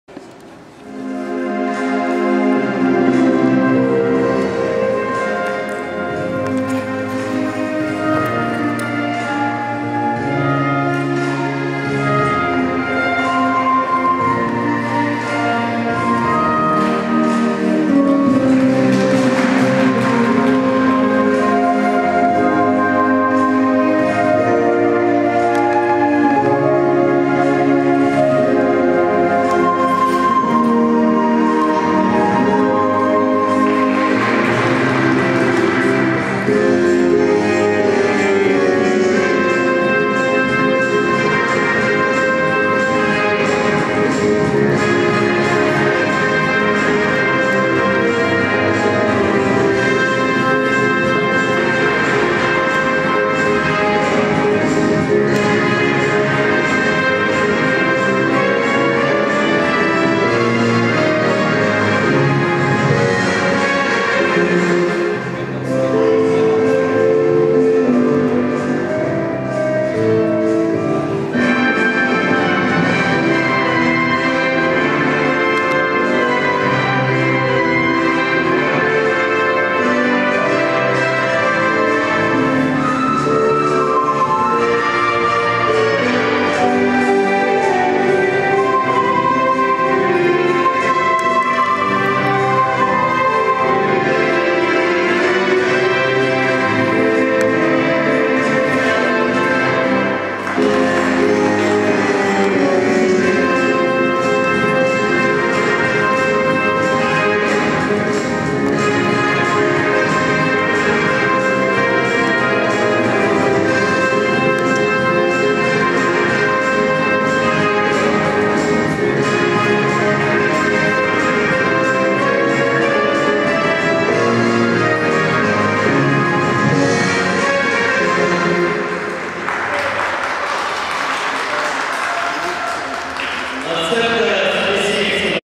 Aleksandra GLAMAZDINA ПП
14 — 19 апреля 2013. Финал Кубка Украины среди юношей г. Харьков.